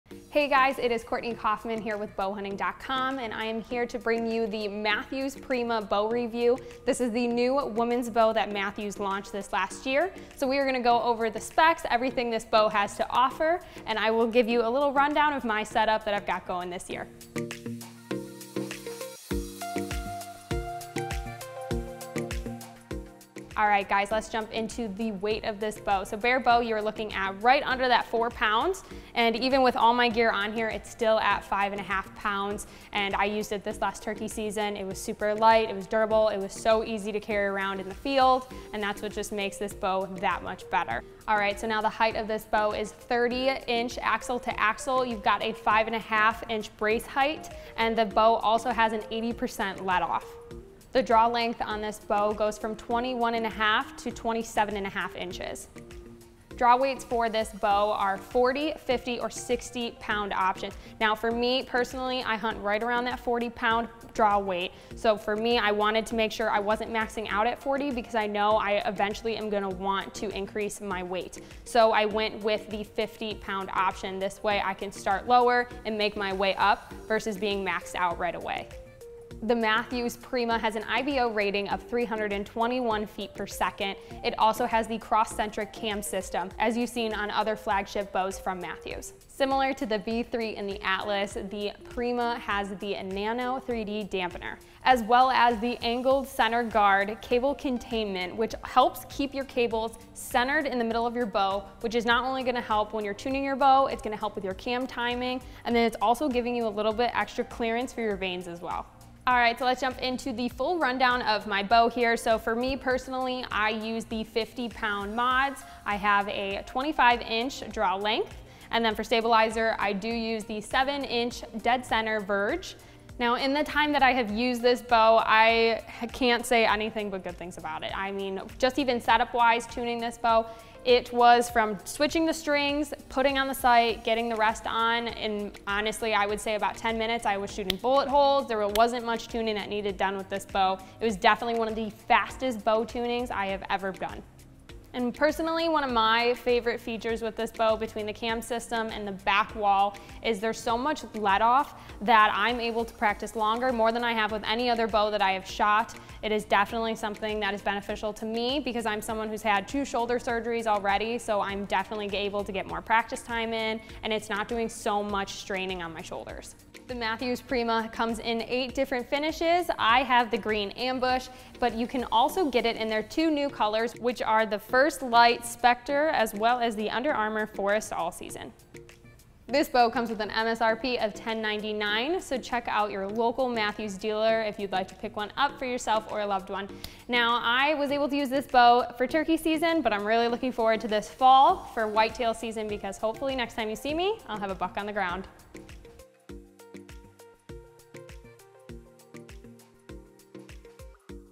Mathews Prima Bow Review
The all-new 2021 Mathews Prima is a great option for both women and men! With a solid back wall and dead-in-the-hand feeling, this is a great shooting bow. It's got the same Crossentric cam system as their other flagship bows but in a slightly smaller package. Here's a look at the new Mathews Prima...

BOW SPECS:
Weight - 3.93lb
ATA Length - 30"
Brace Height - 5 1/2"
IBO Speed - 321fps (at 27.5")
Draw Weights - 40, 50, 60lb
Draw Lengths - 21.5" - 27.5"
Let-Off - 80%
MSRP - $1,099

ABB Platinum Bow String (Electric Blue/Phantom Teal)